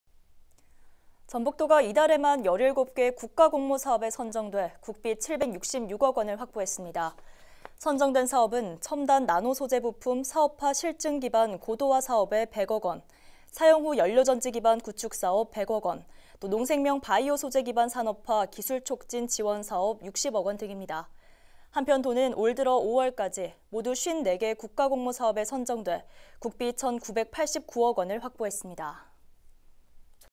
전북도, 이달에 17개 국가공모사업 선정…766억 확보
전북도가 이달에만
17개 국가공모사업에 선정돼
국비 766억 원을 확보했습니다.

선정된 사업은 첨단 나노소재부품
사업화 실증 기반 고도화 사업 100억 원,
사용 후 연료전지 기반 구축사업 100억 원,
농생명 바이오소재 기반 산업화
기술 촉진 지원사업 60억 원 등입니다.

한편 도는 올 들어 5월까지
모두 54개 국가공모사업에 선정돼
국비 1천 989억 원을 확보했습니다.